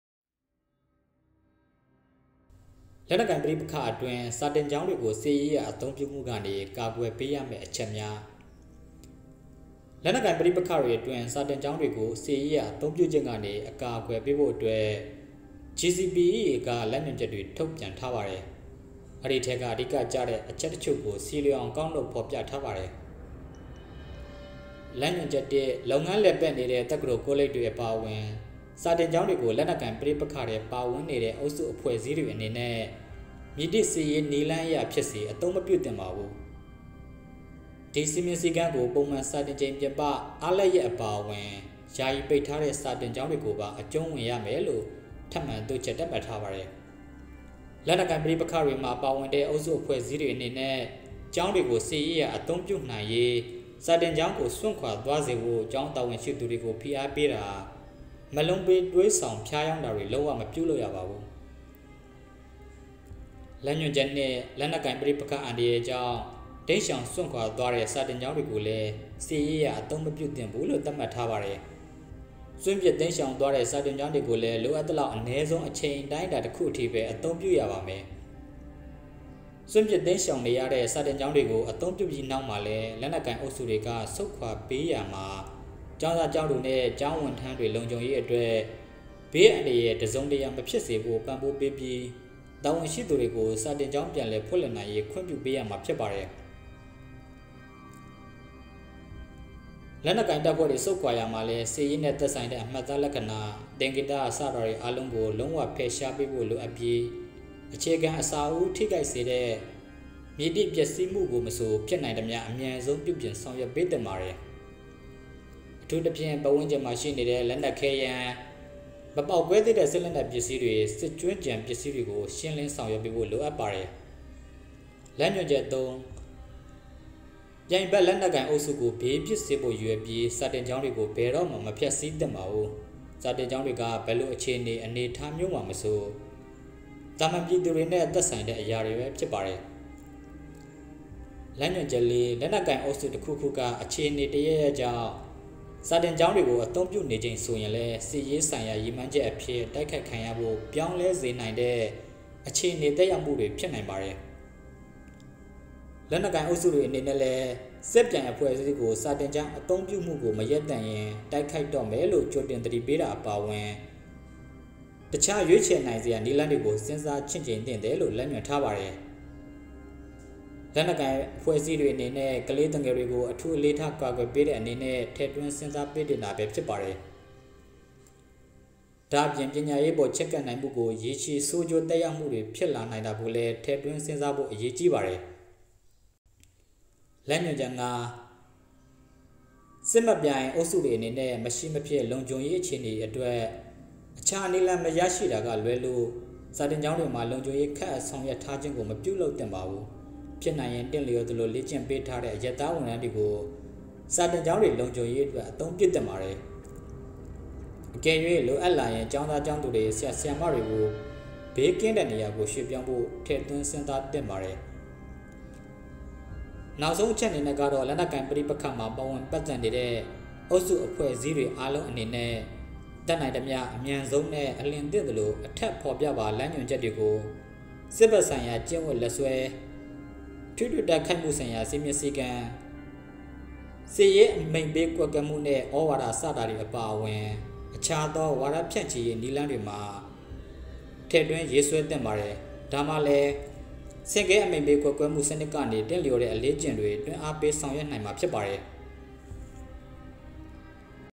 လက်နက်ကိုင် ပဋိပက္ခအတွင်း စာသင်ကျောင်းတွေကို ကာကွယ်ပေးရမယ့် လမ်းညွန်ချက်များ၊
လက်နက်ကိုင် ပဋိပက္ခတွေအတွင်း စာသင်ကျောင်းတွေကို စစ်ရေးအရ အသုံးပြုခြင်းကနေ အကာအကွယ် ပေးဖို့အတွက် GCPEA က လမ်းညွန်ချက်တွေ ထုတ်ပြန်ထားပါတယ်။ အဲဒီထဲက အဓိကကျတဲ့ အချက်တချို့ကို ဆီလျော်အောင် ကောက်နှုတ် ဖော်ပြအပ်ပါတယ်။

(GCPEA ဆိုတာ လက်နက်ကိုင် ပဋိပက္ခတွေအတွင်း စာသင်ကျောင်းတွေကို စစ်ရေးအရ အသုံးပြုခြင်း ပြဿနာကို ဖြေရှင်းနိင်ဖို့အတွက် နိုင်ငံတကာ အေဂျင်စီတွေ အချင်းချင်း ပူးပေါင်းဆောင်ရွက်ဖို့ ၂၀၁၀ မှာ ဖွဲ့စည်းထားတဲ့ အဖွဲ့ ဖြစ်ပါတယ်)

လမ်းညွှန်ချက် (၁) - လုပ်ငန်းလည်ပတ်နေတဲ. တက္ကသိုလ်၊ ကောလိပ်တွေ အပါအ၀င် စာသင်ကျောင်းတွေကို လက်နက်ကိုင် ပဋိပက္ခထဲ ပါ၀င်နေတဲ. အုပ်စု အဖွဲ့အစည်းတွေ အနေနဲ. မည်သည်. စစ်ရေး နည်းလမ်းအရ ဖြစ်စေ အသုံးမပြုသင့်ပါဘူး။
(က) ဒီစည်းမျဉ်းစည်းကမ်းကို ပုံမှန်စာသင်ချိန်ပြင်ပ အားလပ်ရက်အပါအ၀င် ယာယီပိတ်ထားတဲ့. စာသင်ကျောင်းတွေပါ အကျုံး၀င်ရမယ်လို. ထပ်မံတိုးချဲ. သတ်မှတ်ထားပါတယ်။
(ခ) လက်နက်ကိုင် ပဋိပက္ခမှာ ပါ၀င်တဲ. အုပ်စုအဖွဲ့အစည်းတွေ အနေနဲ. ကျောင်းတွေကို စစ်ရေးအရ အသုံးပြုနိုင်ရေး စာသင်ကျောင်းကို စွန်.ခွာသွားစေဖို့ ကျောင်းတာ၀န်ရှိသူ တွေကို ဖိအားပေးတာ၊ မက်လုံးပေး သွေးဆောင် ဖျားယောင်းတာတွေ လုံး၀ မပြုလုပ်ရပါဘူး။

လမ်းညွှန်ချက် (၂) - လက်နက်ကိုင်ပဋိပက္ခ အန္တရာယ်ကြောင့် တိမ်းရှောင်စွန်.ခွာသွားတဲ့ စာသင်ကျောင်းတွေကိုလည်း စစ်ရေးအရ အသုံးမပြုသင့်ဘူးလို. သတ်မှတ်ထားပါတယ်။

စွန်.ပစ်တိမ်းရှောင်သွားတဲ. စာသင်ကျောင်းတွေကိုလည်း လိုအပ်သလောက် အနည်းဆုံး အချိန်အတိုင်းအတာ တစ်ခုအထိပဲ အသုံးပြုရပါမယ်။ စွန်.ပစ်တိမ်းရှောင်နေရတဲ. စာသင်ကျောင်းတွေကို အသုံးပြုပြီးနောက်မှာလည်း လက်နက်ကိုင် အုပ်စုတွေက ဆုတ်ခွာပေးရာမှာ ကျောင်းသူ၊ကျောင်းသားနဲ. ကျောင်း၀န်ထမ်းတွေ လုံခြုံးရေးအတွက် ဘေးအန္တရာယ် တစုံတရာ မဖြစ်စေဖို. ပံ.ပိုးပေးပြီး တာ၀န်ရှိသူတွေကို စာသင်ကျောင်း ပြန်လည်ဖွင့်လှစ်နိုင်ရေး ခွင့်ပြုပေးရမှာ ဖြစ်ပါတယ်။
လက်နက်ကိုင်တပ်ဖွဲ.တွေ ဆုတ်ခွာရာမှာလည်း စစ်ရေးနဲ.သက်ဆိုင်တဲ. အမှတ်အသားလက္ခဏာ၊ သကေင်္တ စတာတွေ အားလုံးကို လုံး၀ ဖယ်ရှားပေးဖို. လိုအပ်ပြီး အခြေခံအဆောက်အဦး ထိခိုက်စေတဲ. မည်သည်. ပျက်စီးမှုကို မဆို ဖြစ်နိုင်သမျှ အမြန်ဆုံး ပြုပြင်ဆောင်ရွက်ပေးသင့်ပါတယ်။ အထူးသဖြင့် ပတ်၀န်းကျင်မှာ ရှိနေတဲ. လက်နက်ခဲယမ်း၊ မပေါ်က်ကွဲသေးတဲ. စစ်လက်နက်ပစ္စည်းတွေ၊ စစ်ကြွင်းကျန် ပစ္စည်းတွေကို ရှင်းလင်း ဆောင်ရွက်ပေးဖို့ လိုအပ်ပါတယ်။

လမ်းညွှန်ချက် (၃) - ပြိုင်ဖက်လက်နက်ကိုင်အုပ်စုကို ဘေးဖြစ်စေဖို. ရည်ရွယ်ပြီး စာသင်ကျောင်းတွေကို ဘယ်တော.မှ မဖျက်ဆီးသင့်ပါဘူး၊ စာသင်ကျောင်းတွေက ဘယ်လိုအခြေအနေ၊ အနေအထားမျိုးမှာမဆို သာမန်ပြည်သူတွေနဲ. သက်ဆိုင်တဲ. အရာတွေပဲ ဖြစ်ပါတယ်။

လမ်းညွှန်ချက် (၄) - လက်နက်ကိုင်အုပ်စု တစ်ခုခုက အခြေအနေ တစ်ရပ်ရပ်ကြောင့် စာသင်ကျောင်းတွေကို အသုံးပြုနေချိန်ဆိုရင်လည်း စစ်ရေးဆိုင်ရာ ရည်မှန်းချက်အဖြစ် တိုက်ခိုက်ခံရဖို. ပြောင်းလဲ စေနိုင်တဲ. အခြေအနေ သက်ရောက်မူတွေ ဖြစ်နိုင်ပါတယ်။ လက်နက်ကိုင်အုပ်စုတွေ အနေနဲ.လည်း စစ်ပြိုင် အဖွဲ.အစည်းကို စာသင်ကျောင်း အသုံးပြုမူကို မရပ်တန်.ရင် တိုက်ခိုက်တော.မယ်လို့ ကြိုတင်သတိပေးတာ အပါအ၀င် တခြားရွေးချယ်နိုင်စရာ နည်းလမ်းတွေကို စဉ်းစားချင့်ချိန်သင့်တယ်လို. လမ်းညွန်ထားပါတယ်။ လက်နက်ကိုင် အဖွဲ.အစည်းတွေအနေနဲ. ကလေးသူငယ်တွေကို အထူးအေလးထား ကာကွယ်ပေးတဲ. အနေနဲ့ ထည့်သွင်းစဉ်းစားပေးသင့်တာပဲ ဖြစ်ပါတယ်။ ဒါ.အပြင် ပညာရေးအပေါ် ချည်းကပ်နိုင်မူကို ရေရှည်ဆိုးကျိုး သက်ရောက်မူတွေ ဖြစ်လာနိုင်တာကိုလည်း ထည့်သွင်းစဉ်းစားဖို. အရေးကြီးပါတယ်။

လမ်းညွန်ချက် (၅) - စစ်မက်ပြိုင် အုပ်စုတွေ အနေနဲ. မရှိမဖြစ် လုံခြုံးရေးအခြေအနေအတွက် အခြားနည်းလမ်း မရရှိတာကလွဲလို. စာသင်ကျောင်းတွေမှာ လုံခြုံရေး ခန်.အပ်ဆောင်ရွက်ထားခြင်းကို မပြုလုပ်သင့်ပါဘူး။ ဖြစ်နိုင်ရင် သင့်လျော်သလို လေ.ကျင့်ပေးထားတဲ. အရပ်သား ၀န်ထမ်းတွေကို စာသင်ကျောင်းတွေ လုံခြုံးရေးအတွက် အသုံးပြုသင့်ပါတယ်။ အကယ်၍ လိုအပ်လာရင် ကျောင်းသား၊ကျောင်းသူတွေ၊ ဆရာ၊ဆရာမတွေကို ဘေးကင်းတဲ.နေရာကို ရွှေ.ပြောင်းဖို. ထည့်သွင်းစဉ်းစားသင့်ပါတယ်။

နောက်ဆုံး အချက်အနေနဲ.ကတော့ လက်နက်ကိုင် ပဋိပက္ခမှာ ပါဝင် ပတ်သတ်နေတဲ. အုပ်စု အဖွဲ့အစည်းတွေ အားလုံးအနေနဲ. တက်နိုင်သမျှ အမြန်ဆုံး၊နဲ.အလျည်းသင့်သလို အထက်ဖော်ပြပါ လမ်းညွန်ချက်တွေကို စစ်ဘက်ဆိုင်ရာ ကျင့်၀တ်လက်စွဲ၊ ထိတွေ.တိုက်ခိုက်မူဆိုင်ရာ စည်းမျည်းစည်းကမ်း၊ စစ်ဆင်ရေး အမိန်.ပေး ကွပ်ကဲမူနဲ. သြဝါဒ စတာတွေ အပါအ၀င် အခြားသော ဝါဒဖြန်.ချိရေး နည်းလမ်းတွေမှာ ထည့်သွင်း ရေးဆွဲသင့်ပါတယ်။ ဒါမှလည်း ဆင့်ကဲအမိန်.ပေး ကွပ်ကဲမူစနစ်ကနေ သင့်လျော်တဲ့ အလေ့အကျင့်တွေ တွန်းအားပေး ဆောင်ရွက်နိုင်မှာ ဖြစ်ပါတယ်။